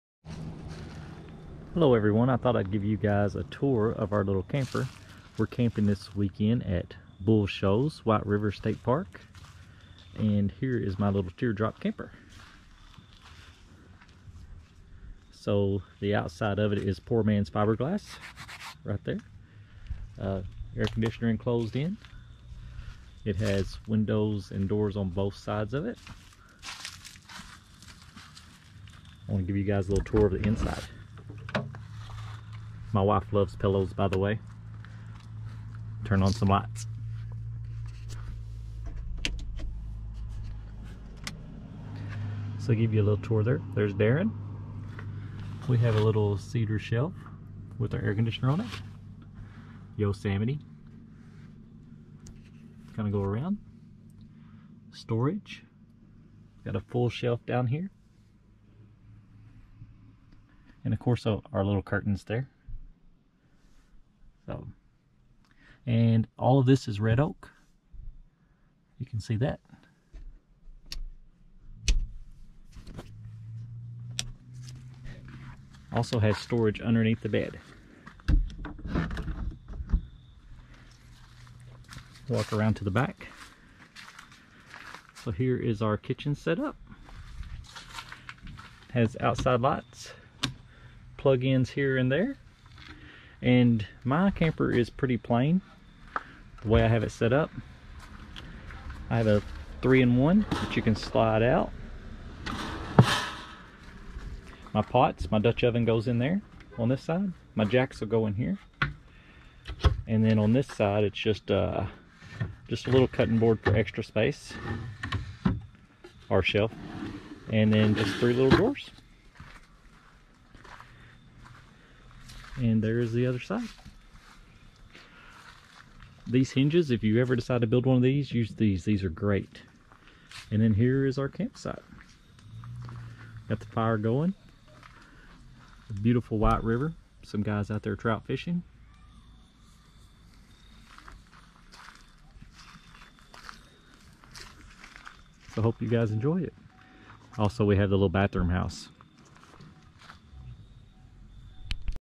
My teardrop trailer build
My teardrop trailer is built on a harbor freight trailer.  The camper is 5x8.  I hope that you enjoy it!